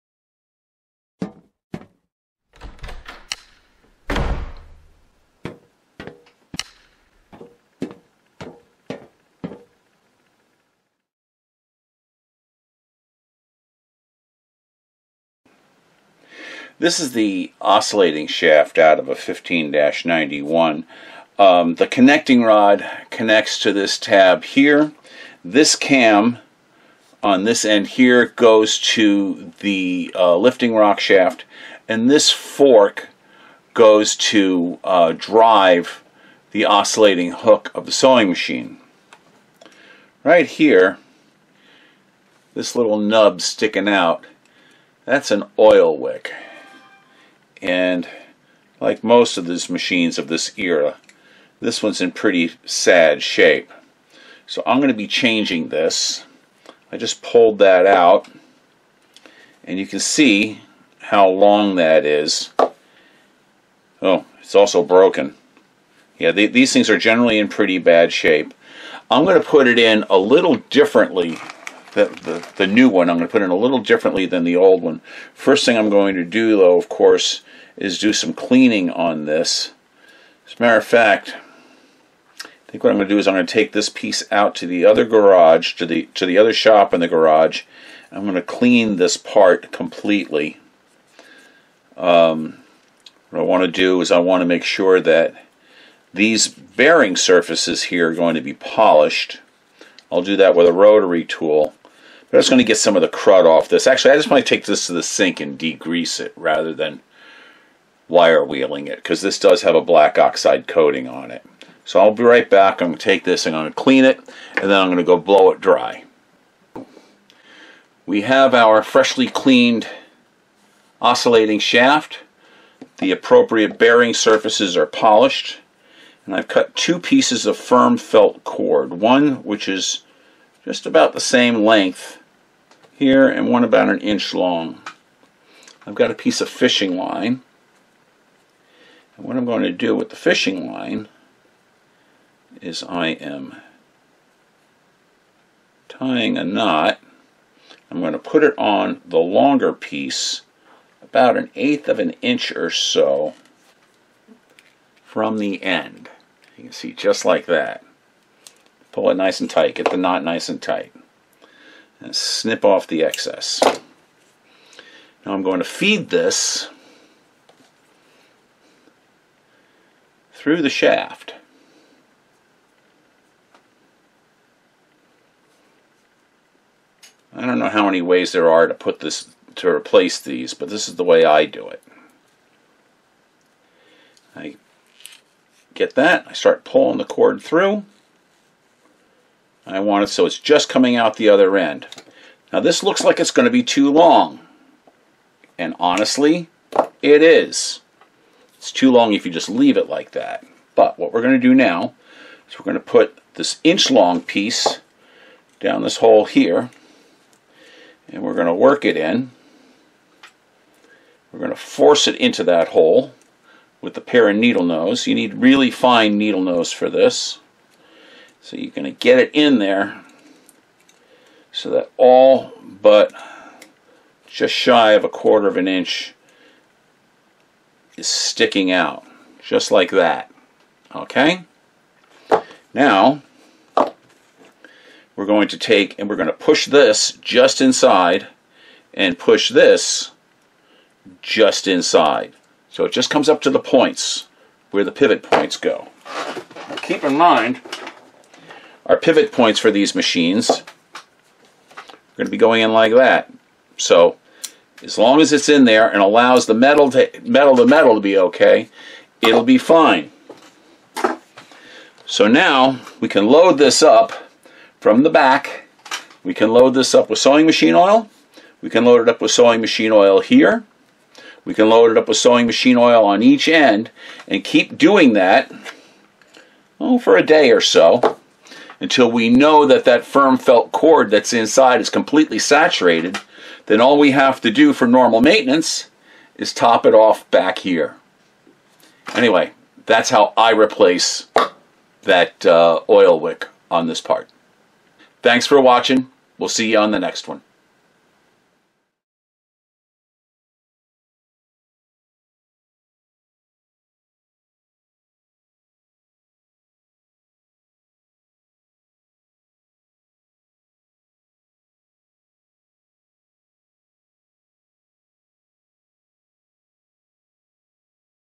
Singer 15 91: Oscillating Shaft OIl Wick Replacement
Did you know that there is an oil wick on the underside of the Singer 15-91, and it's part of the oscillating hook mechanism? A lot of people don't know it's there, much less how to replace it when it gets old and crusty. Here's how I do the job...

Oh yes... I use 1/8" firm felt cord from McMaster-Carr for the wick material. You can find a link to them in the Suppliers list at my website below.

The feature on my web page - "My Recommended Tools, Materials, and Suppliers" can be found here:

Include lots of pictures if possible and let me know the gory details!